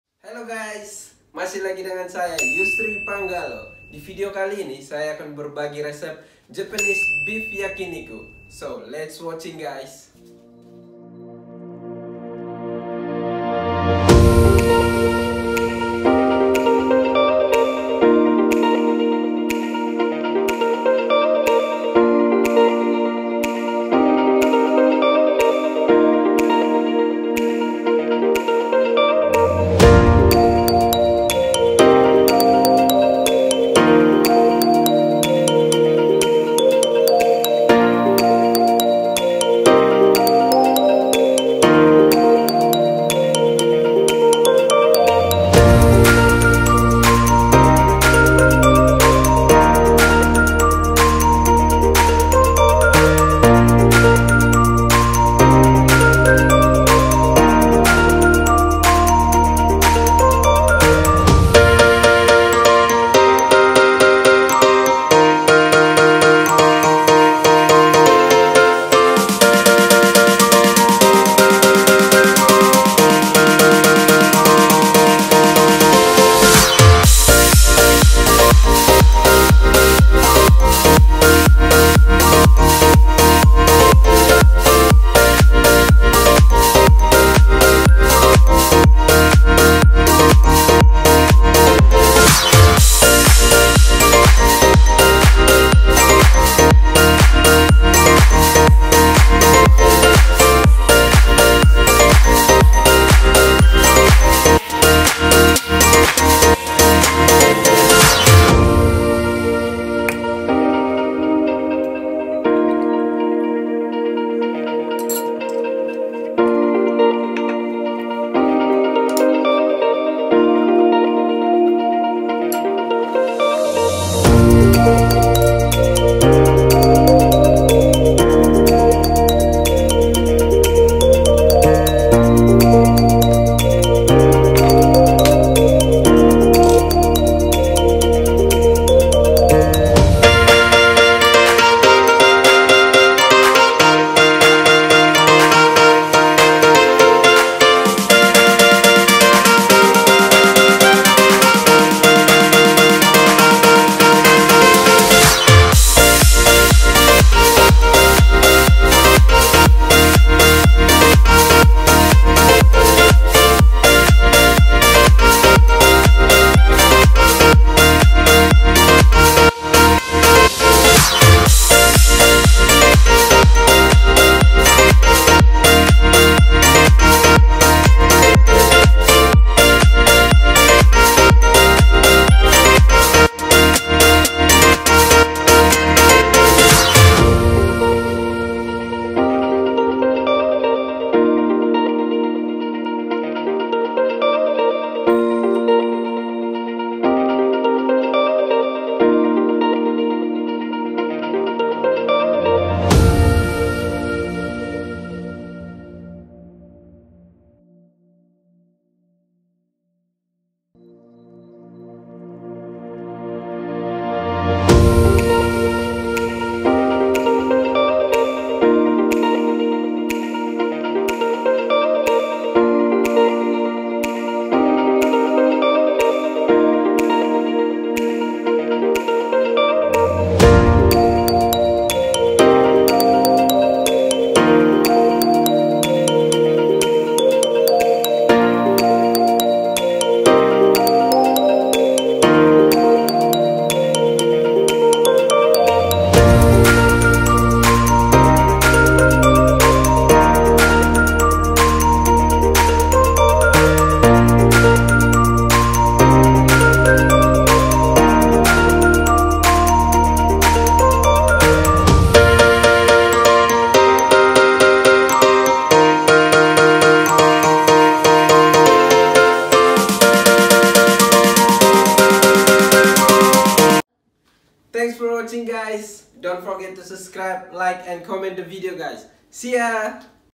JAPANESE BEEF YAKINIKU - Simple and Easy
Recipe
- 300g sliced beef
- 2Tbs soy sauce
-2Tbs Sake
-2 Tbs honey
-1Tbs Oyster Sauce
-1Tbs sesame Oil
-Half browm onion
- 1 Spring oniom